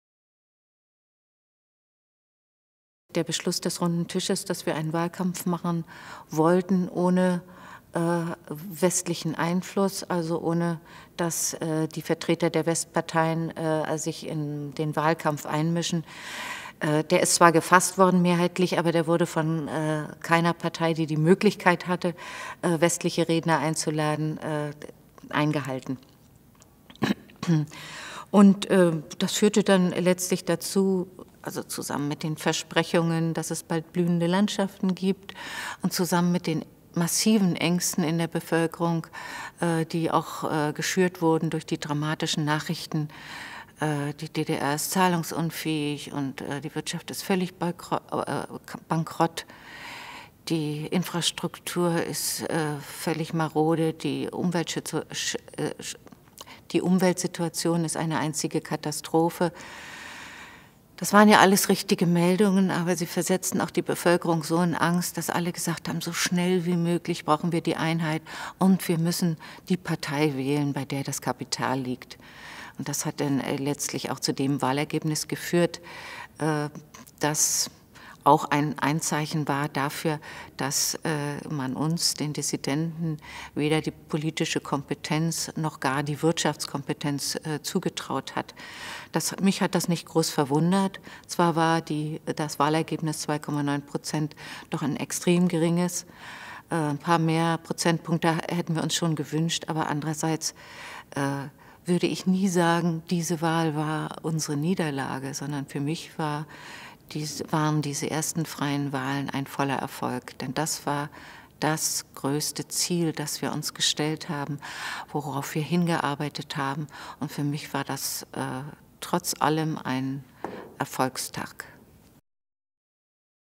Ulrike Poppe: Volkskammerwahl, 18. März 1990
Die 1953 geborene Bürgerrechtlerin und Oppositionelle Ulrike Poppe tritt mit ihrer Gruppe „Demokratie jetzt“ bei den ersten freien Wahlen in der DDR an. Sie erinnert sich an die Gründe für die schlechten Wahlergebnisse ihrer Gruppe.